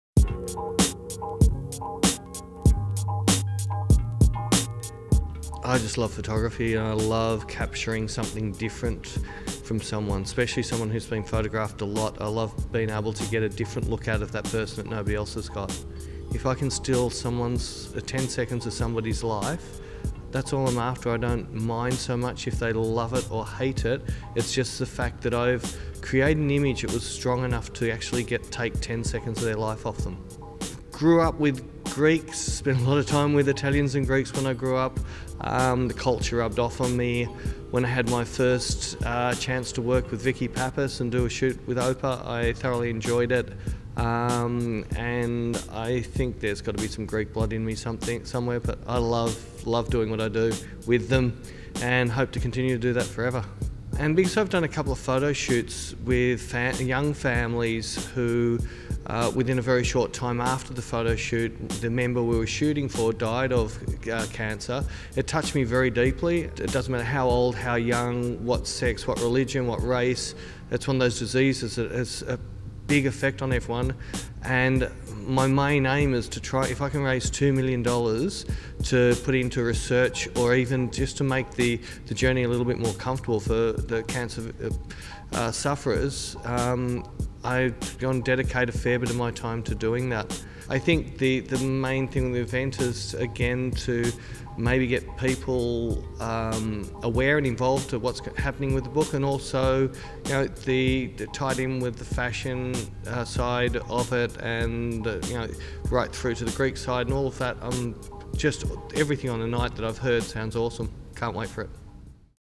Peter Coulson :: MODA Cancer Fundraiser 2012 :: OPA Magazine
Photographer Peter Coulson talks about the MODA Fashion fundraiser event and his upcoming fundraising photo book 'In My Pants'.